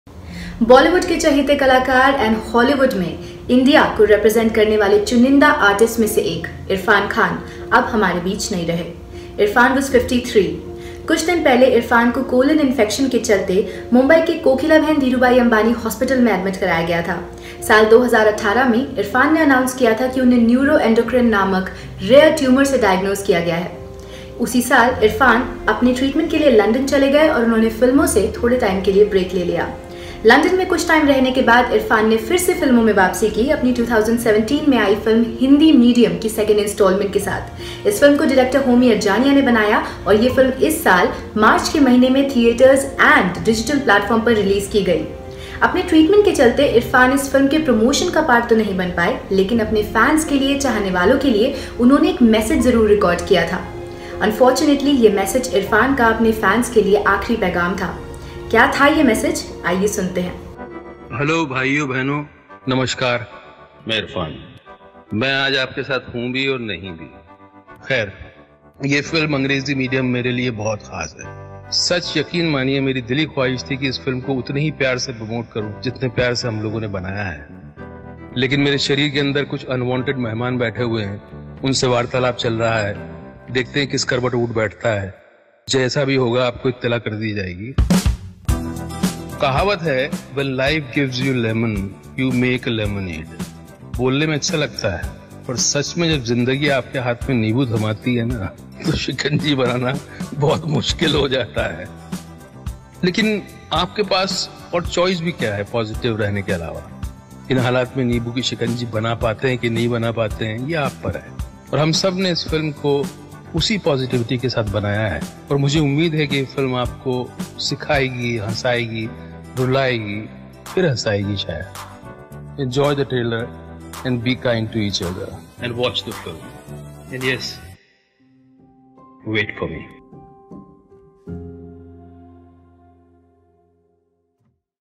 RED FM: 'Slumdog Millionaire' Actor Irrfan Khan Dies At 53
Indian actor Irrfan Khan dies at 53 due to a rare neuroendocrine tumor.